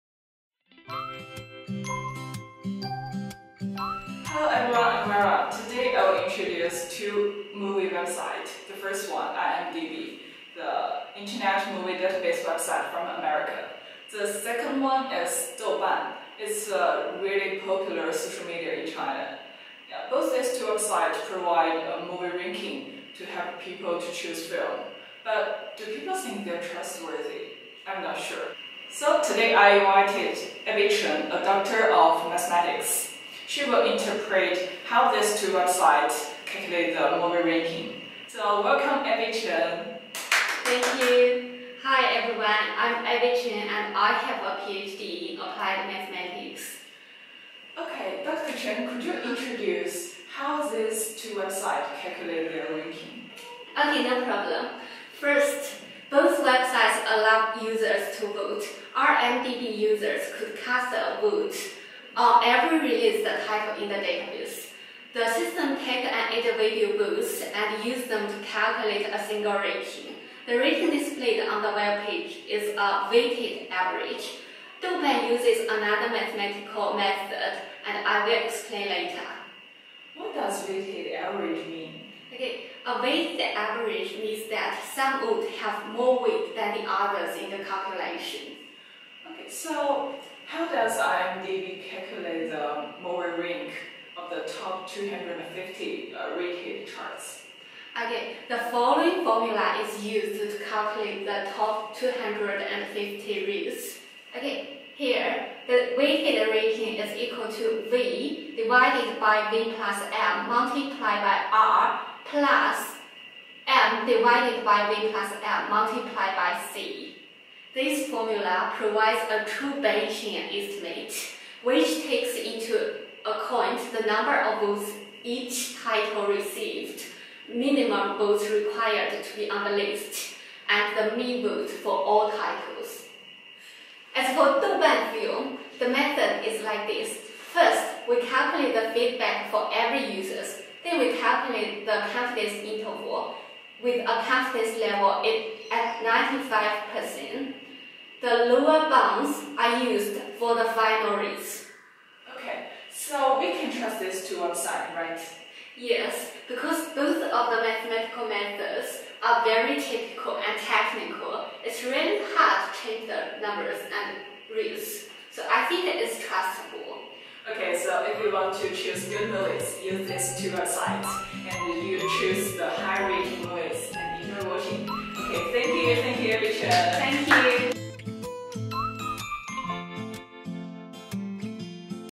IMDb vs. Douban Film - Interview
This is a interview about the mathematical methods used by the movie rating systems of IMDb and Douban Film. The interviewee, Abby Chen, is a Doctor of Mathematics, graduate from the Swinburne University of Technology.